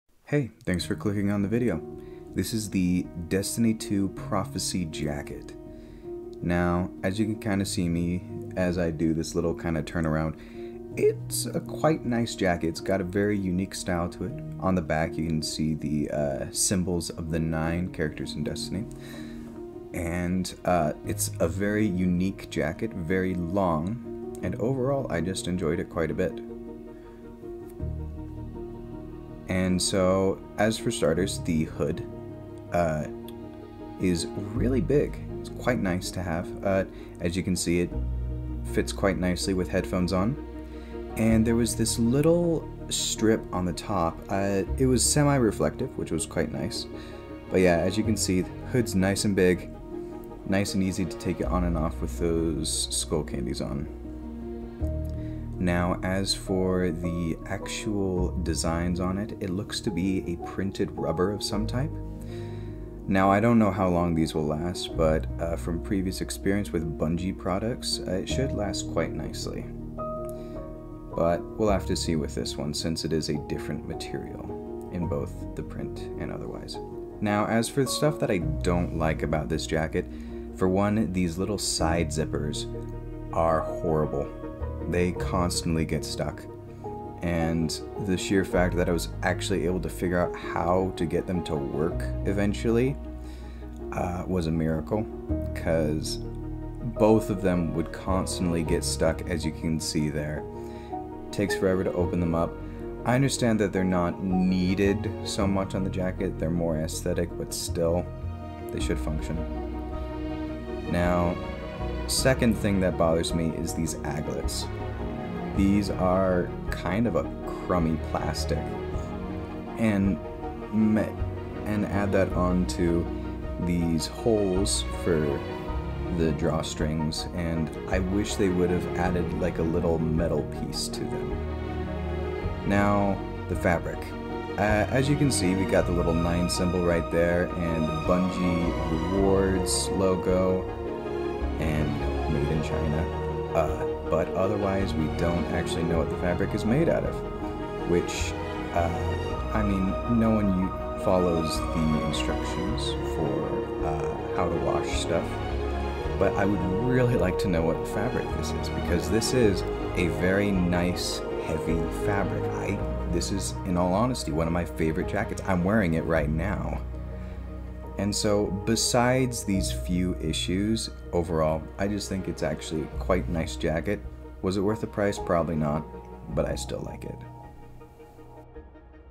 Prophesy Hoodie (Destiny 2)
Just thought I would start with the latest collection of my wardrobe of nerdy clothes, especially since it is very, very nice, sadly it is no longer available from Bungie anymore.